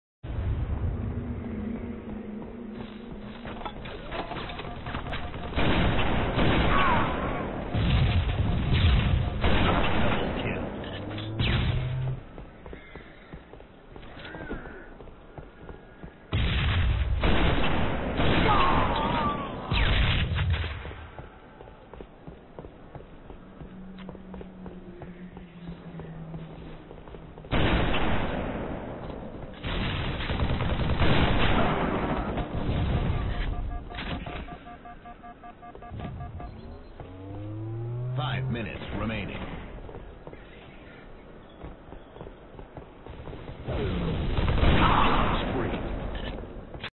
noscopehead x 5
GamerTag: sardinebox123 / Film Name: noscopehead x 5
Description: cornered with no shield & sniper = 4 no scopes in head & killing spree
Slot: 1